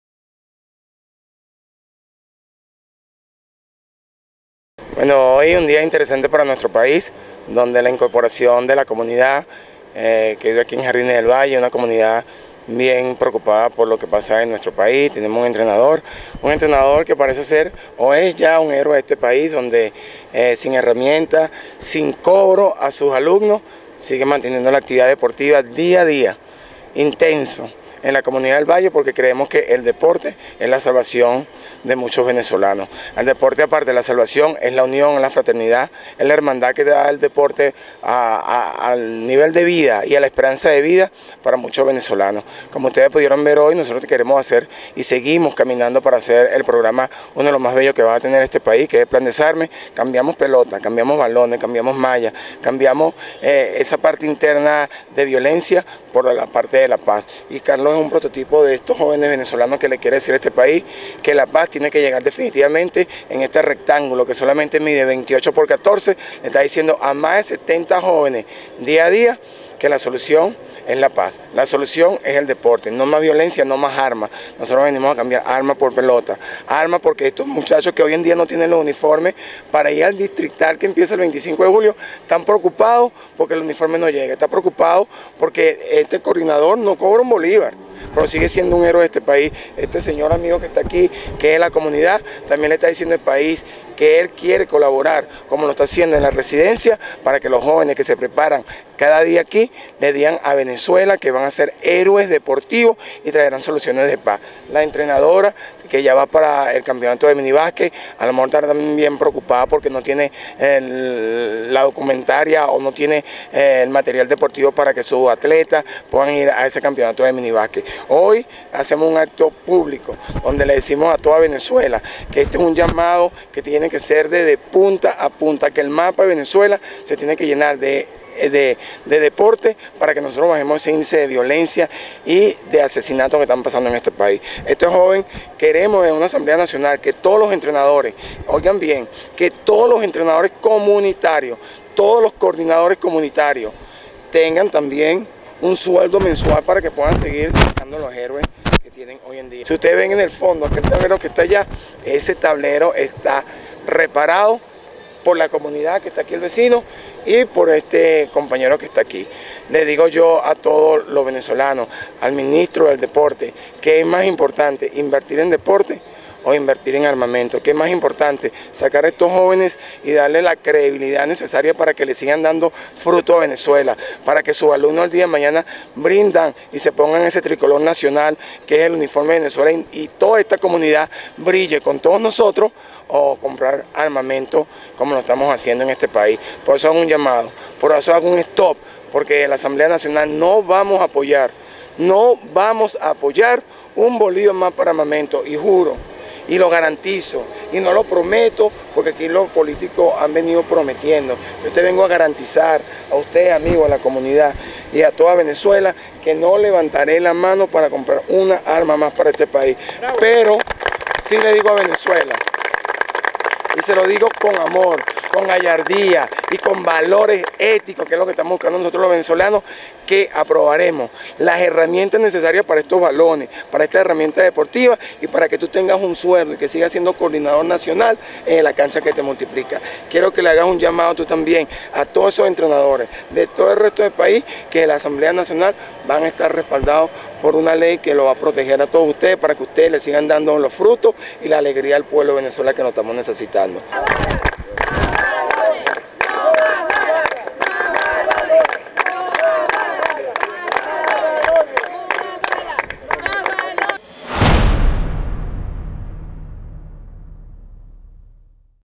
Tolda amarilla incentiva actividades de deporte y esparcimiento para jóvenes
Yván Olivares: "No levantaré nunca mi mano para aprobar la compra de más armamento militar en el país"

El candidato a la Asamblea Nacional por el circuito 1 del Distrito Capital visitó la comunidad de Jardines de El Valle e hizo entrega de balones a jóvenes que practican en la cancha de los bloques 1 de mayo, Carabobo y Fetratel bajo la premisa que con más recursos para el entrenamiento, los alumnos no se verán tentados a delinquir. Aseguró que en la AN trabajará para la aprobación de la Ley de Desarme y para otorgar derechos sociales y salariales a los entrenadores deportivos.

Caracas, 3 de junio de 2010.-  En su recorrido por las comunidades de Caracas, Yván Olivares, candidato por el circuito 1 a la Asamblea Nacional, se comprometió este jueves con el sector Jardines de El Valle, bloques 1 de mayo, Carabobo y Fetratel, donde se ubica una cancha de baloncesto donde practican más de 60 niños.

Expresó Olivares, en encuentro con entrenadores, jóvenes, vecinos e integrantes de la Juvenil de Primero Justicia, que una de sus propuestas como próximo diputado, es incentivar la aprobación de la Ley de Desarme, donde a través de prácticas deportivas los jóvenes no se vean en la necesidad de delinquir. "Hoy es un día interesante para nuestro país, un entrenador héroe mantiene su actividad en El Valle, pese a las limitaciones". Prometió Olivares que frente al entusiasmo de los entrenadores como Carlos Álvarez, hará una entrega de uniformes y aportes para la mejora de la cancha de la zona, la cual se encuentra sin puerta y con las rejas dañadas. Igualmente hizo entrega este jueves, de varios balones de baloncesto, a fin de que Álvarez y su equipo de niños continúen desarrollándose en la práctica sana y recreativa.

Por su parte, Rómulo González, vecino de la localidad y uno de los encargados de la cancha, apoyó la iniciativa de Olivares y su equipo, ya que manifestó que fuera de horario de clases y debido al deterioro de la cancha, el espacio deportivo es utilizado por personas de mala vida para ingerir licor y otras irregularidades. Agregó que toda acción que busque fortalecer el deporte en el sector, donde hay jóvenes con el interés de actividades de deporte, debe ser respaldado en beneficio de todos.

Olivares, junto a Dinorah Figuera, también candidata a la AN por lista al Distrito Capital, reiteró que desde la Asamblea Nacional no levantará nunca la mano para aprobar la compra de más armamento militar en Venezuela y que por el contrario, espera gestionar un proyecto de ley que reconozca derechos salariales a los entrenadores deportivos, así como beneficios sociales.

Al grito de "no más balas y sí más balones", los integrantes de la Juvenil de la tolda amarilla simularon en la cancha lo que representa la inseguridad y la mortandad que sufren los jóvenes cuando no cuentan con espacios ni recursos para su desarrollo integral y de esparcimiento.

"Aquí tenemos a las dos Venezuela, una donde la mayoría de los muertos son jóvenes en un 80% y otra donde a través de actividades como las que hace Olivares, se puede transformar la realidad y generar menos violencia", expresó Milagros Paz, secretaria nacional juvenil de Primero Justicia. Añadió que debido a la preocupación que genera la compra de armamento militar por parte del Gobierno Nacional (13 mil millones de dólares en 10 años y 5 mil millones en el último año) los jóvenes se suman al llamado general de la sociedad de que se suspendan las compras de esos armamentos y que en su lugar se hagan inversiones en la juventud. "70% de las personas que están en las cárceles son jóvenes entre 18 y 28 años. Es una pérdida lamentable, de sueños de valores", indicó.

Por su parte, Olivares fue enfático y manifestó "le pido a todos los jóvenes de esta comunidad que se integren. Yo no vine a preguntarle al entrenador de qué partido es, yo vine a apoyarlo porque sé que trabaja sin recursos". Agregó el dirigente de Primero Justicia, que el deporte representa de por sí a la familia ya que el tiempo que dura el entrenamiento los niños están en conjunto con su familia.

Apuntó que todas estas actividades de entrega de balones, en lugar del uso de armas en las comunidades, es una de las alternativas para generar paz y erradicar la violencia de Venezuela. Eso es Justicia", finalizó.